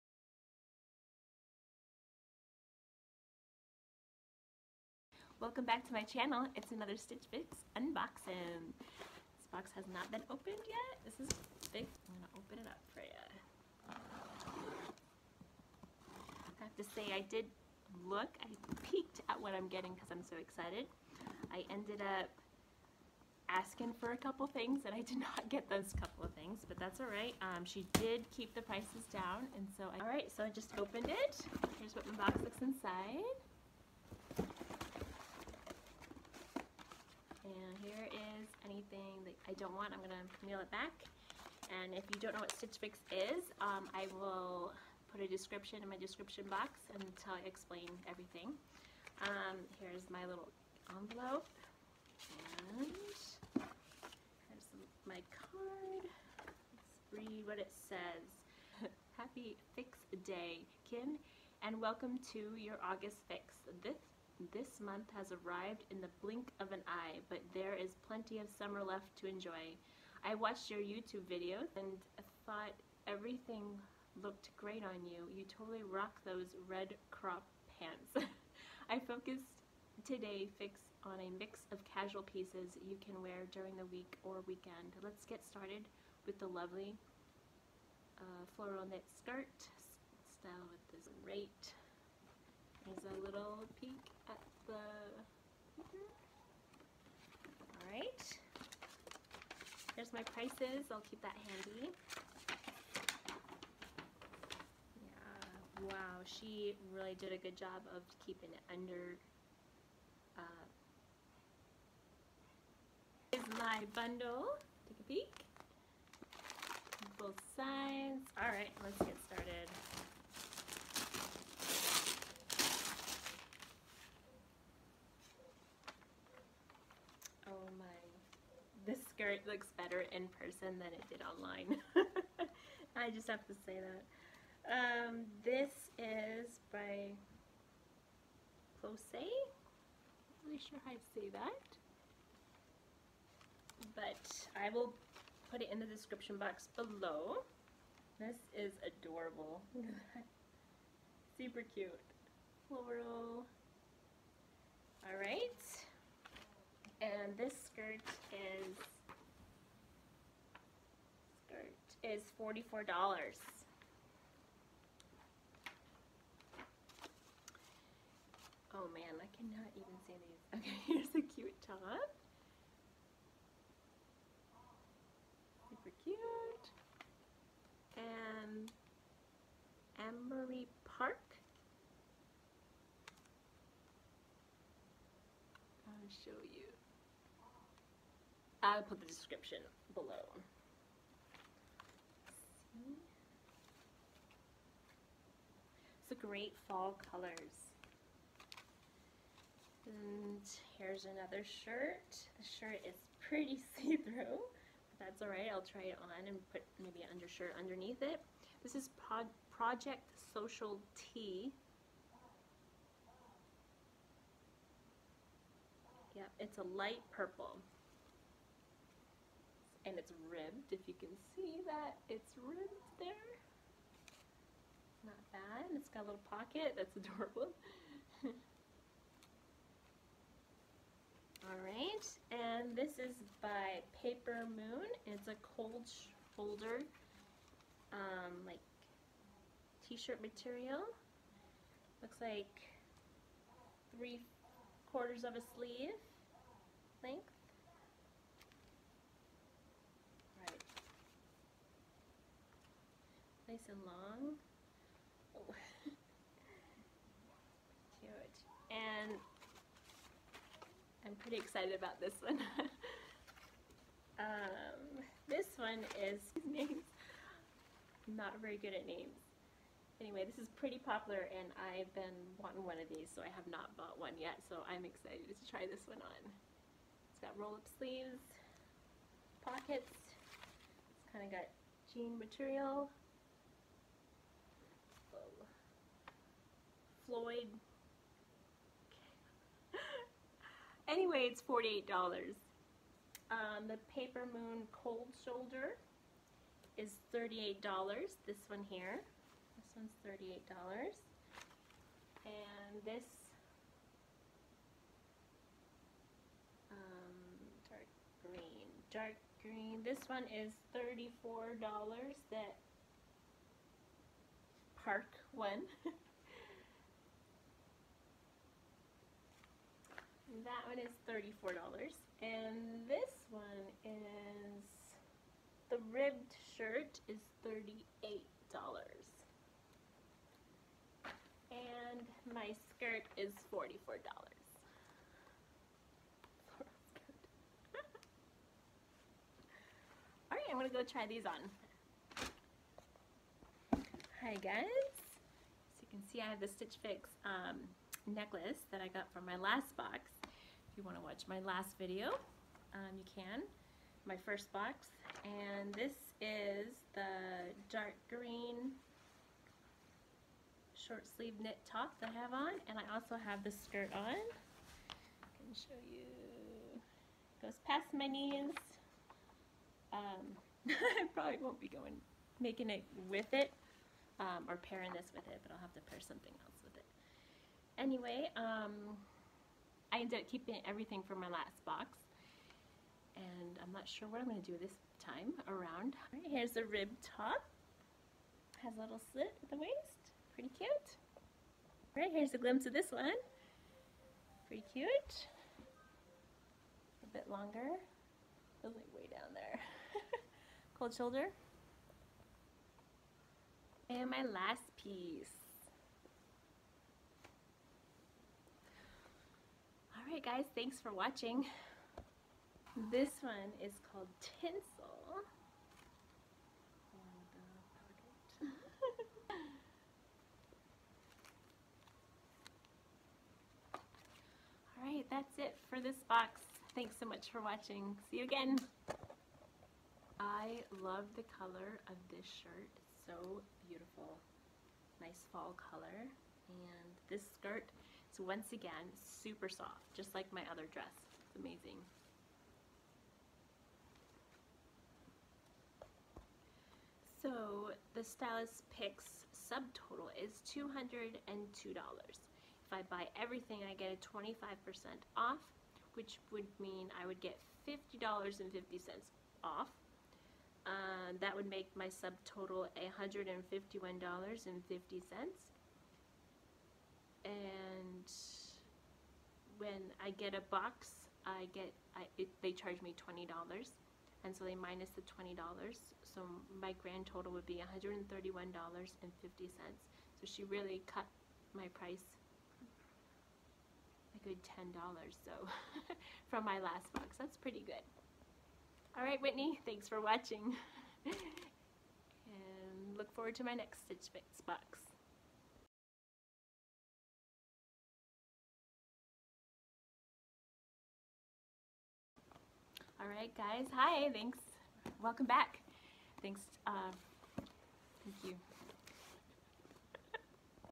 August Stitch Fix unboxing #2 Blooper at the end! 😂
Colette- Mali Brushed Knit Skirt                        $44
Emory Park- Dulce Short sleeve knit top         $34
Project Social T- Aalia Ribbed Hi-low Knit       $38
Papermoon- Aubrey Cold Shoulder Knit          $38
Tinsel- Floid Chambray Top                               $48

I bought the skirt, the Emory Park dark green shirt, and the Tinsel.

God bless!